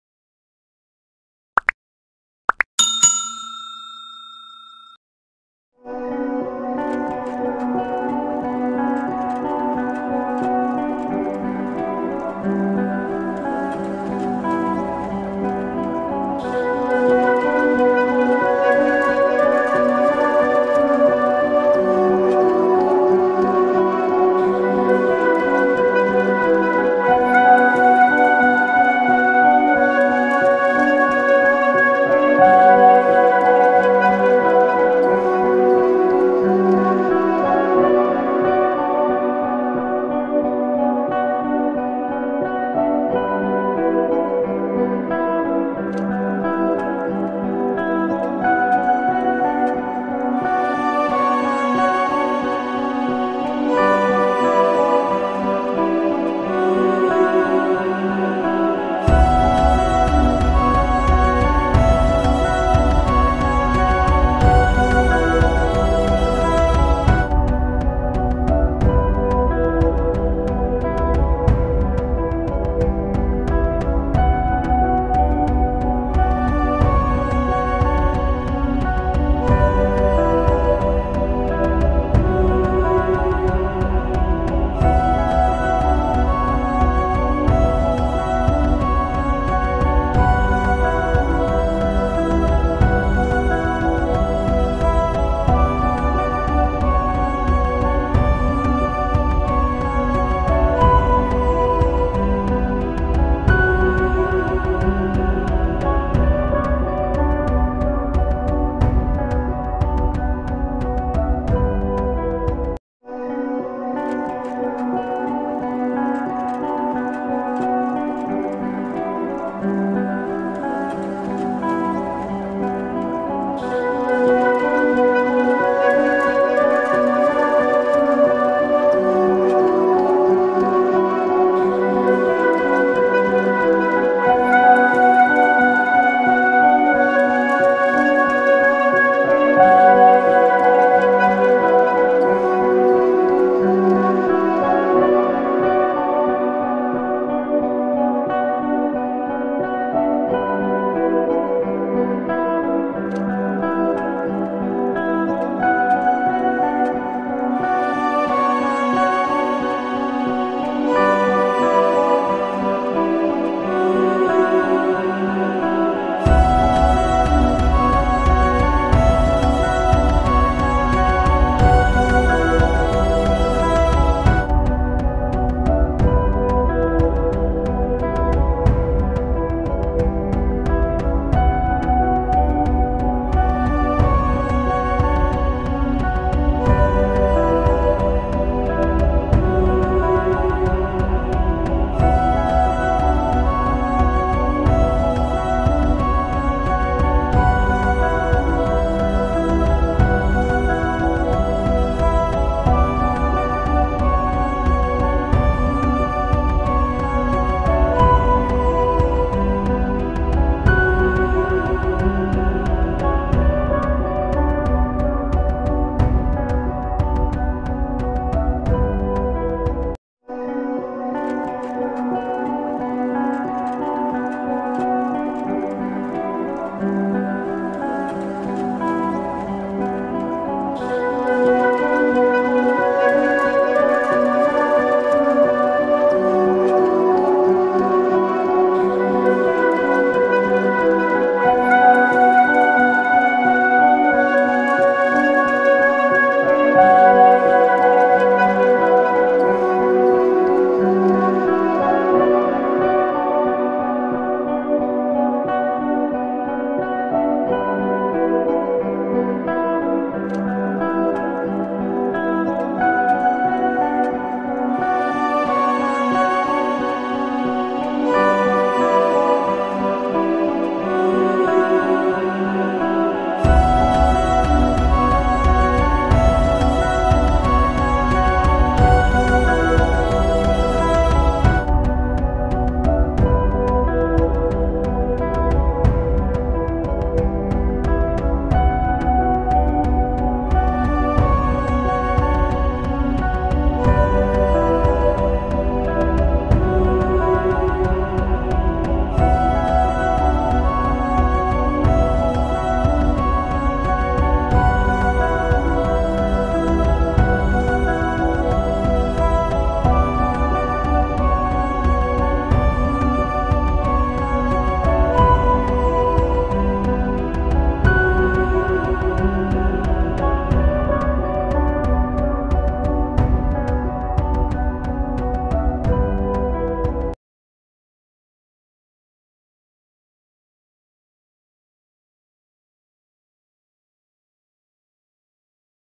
Soft mellow ukulele copyright free background music!!
This video is "always copyright free" youtube channel video. This video provide copyright free background music & it's fre use all purpose. It's Soft mellow ukilele music.